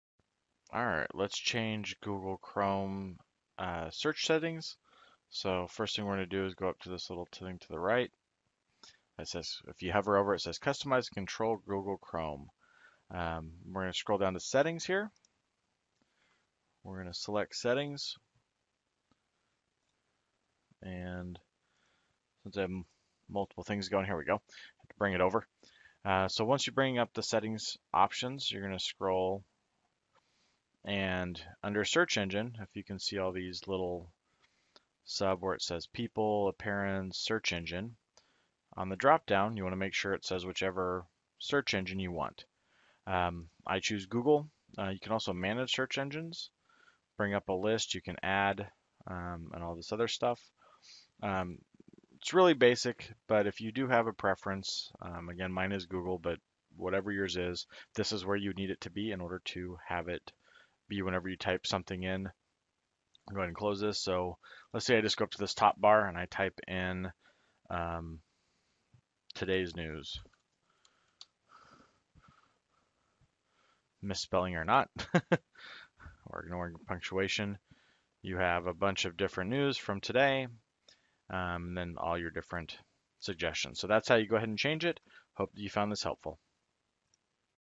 Chrome - change Default Search
This tutorial will show you how to change the default search option for the chrome browser.